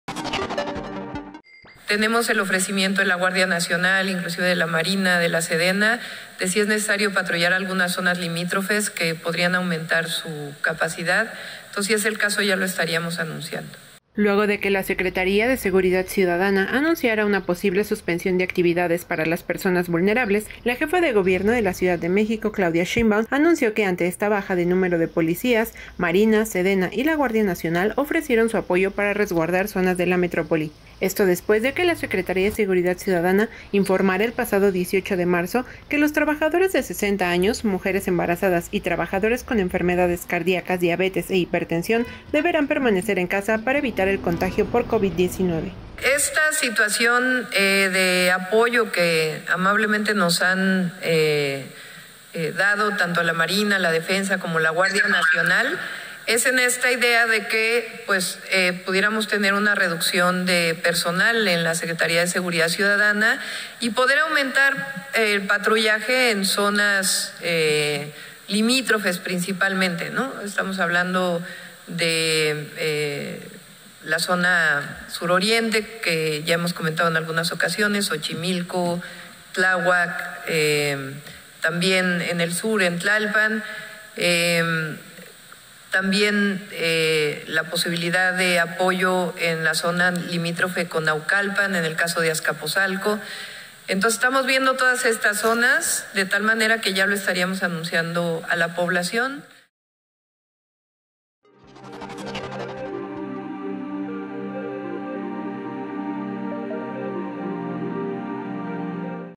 Ante reducción de policías, Marina, Sedena y GN resguardarán la CDMX: Sheinbaum
Esto, luego de anunciarse que policías mayores de 60 años, mujeres embarazadas y quienes padezcan diabetes, hipertensión arterial y enfermedades cardiacas, deberán permanecer en casa para protegerlos del Covid-19